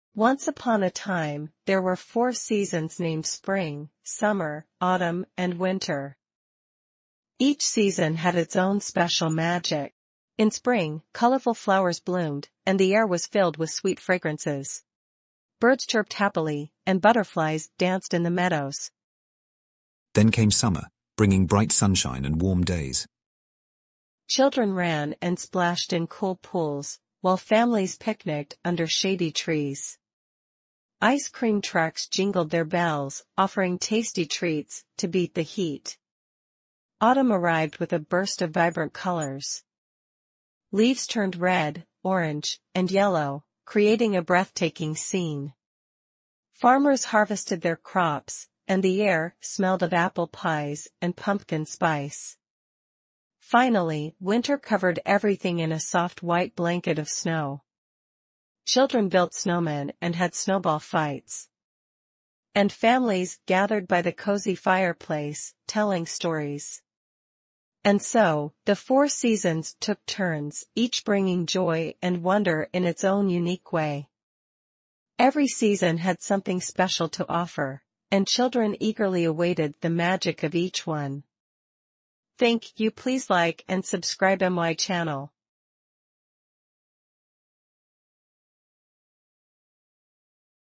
FOUR SEASONS ARE BLESSING
This is an informative cartoon that describes about seasons and their significant